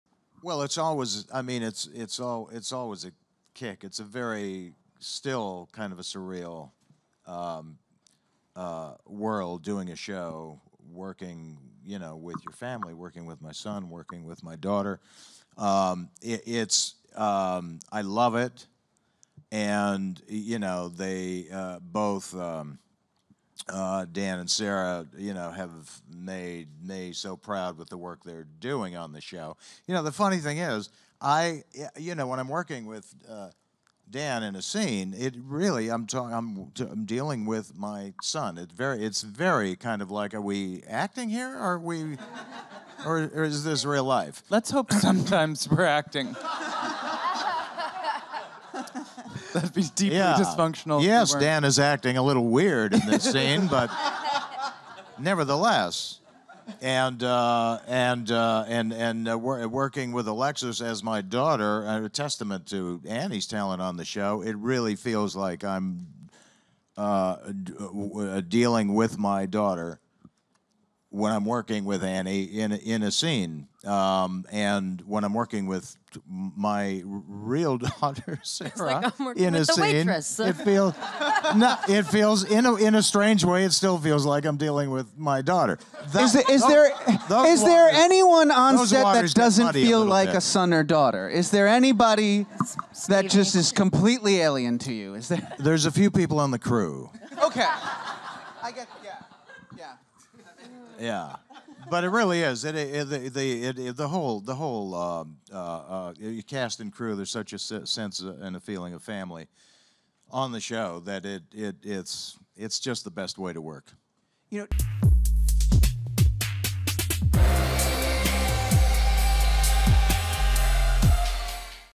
What It's Like Working With Family On "Schitt's Creek"
Eugene Levy, star of "Schitt's Creek," talks about how much he enjoys working with his family on the set of the series.